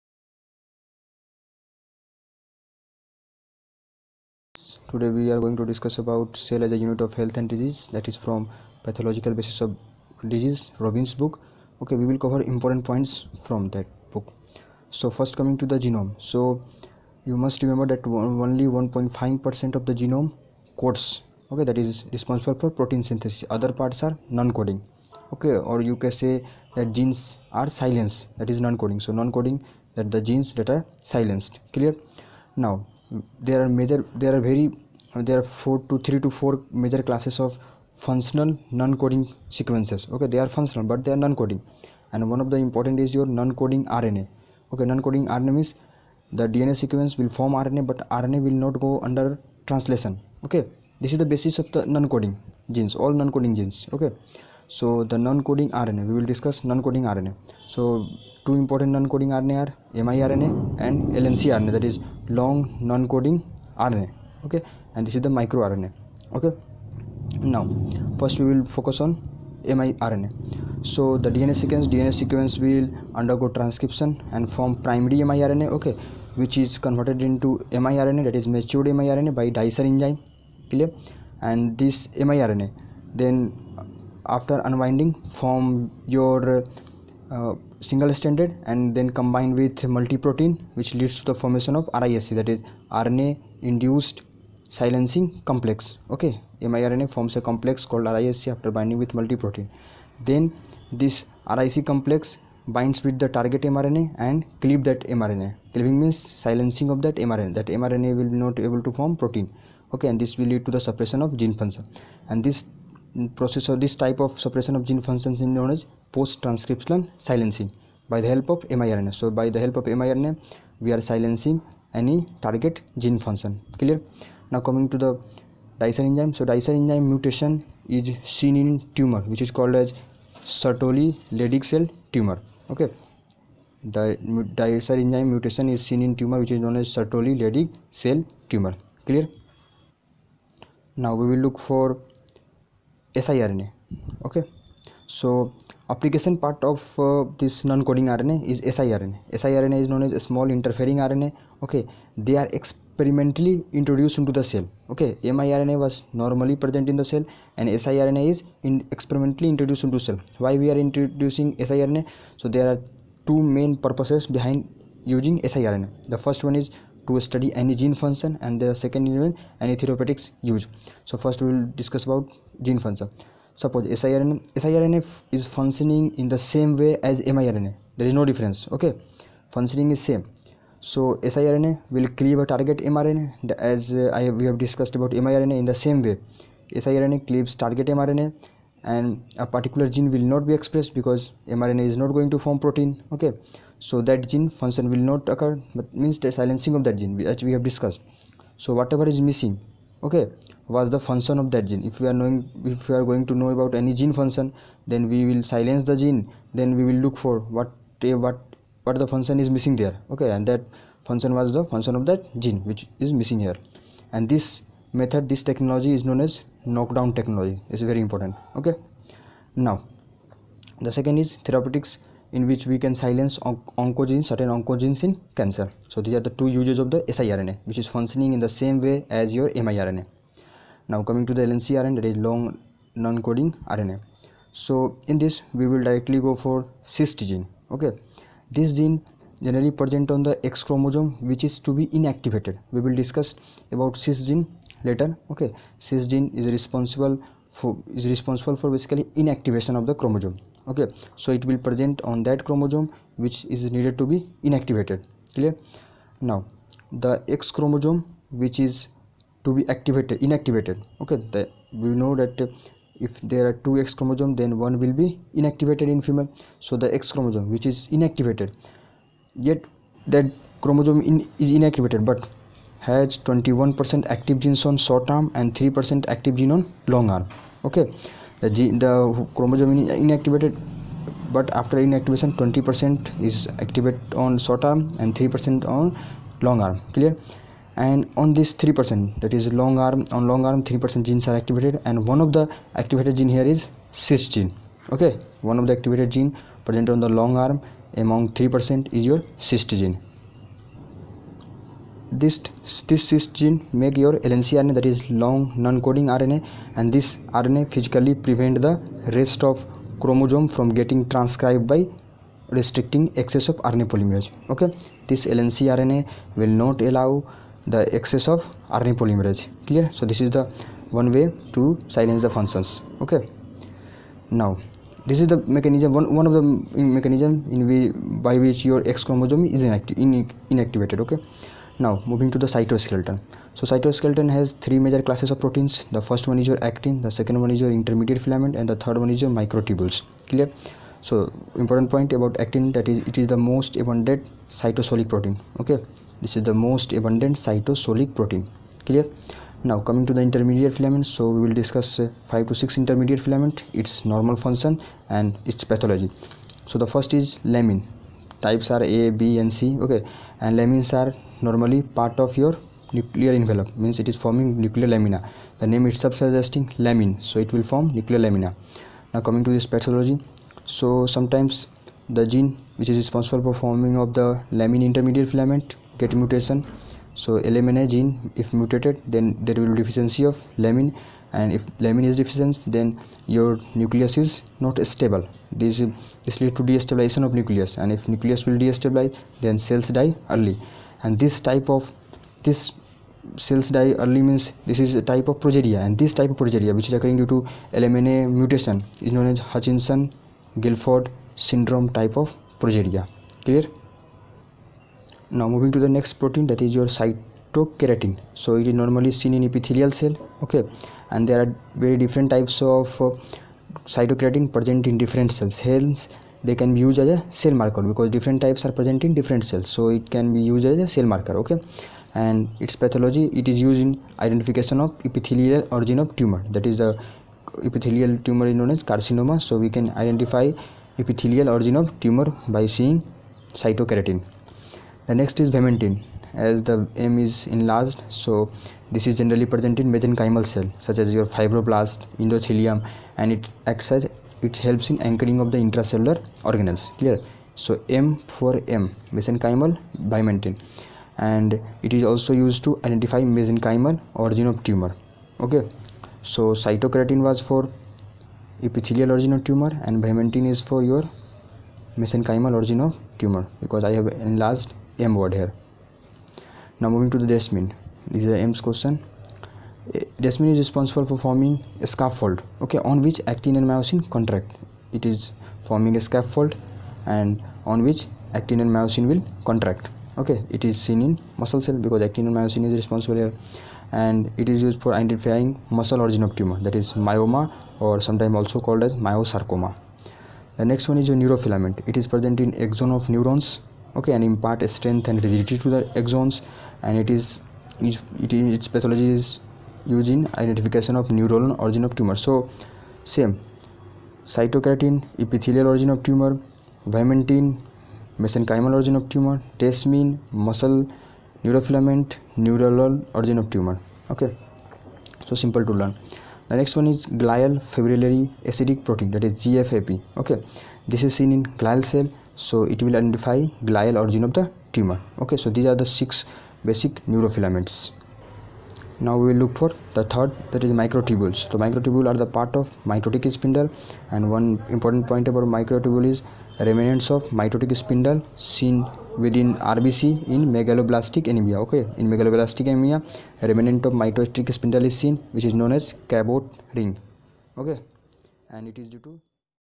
CELL AS A UNIT OF HEALTH AND DISEASE( ROBBINS PATHOLOGY REVIEW VIDEO-DAMS COACHING)PART-1
EXCLUSIVELY FOR MBBS STUDENTS- FOR USMLE and NEET-PG.
HELPFUL FOR YOUR SEMESTER EXAMINATION..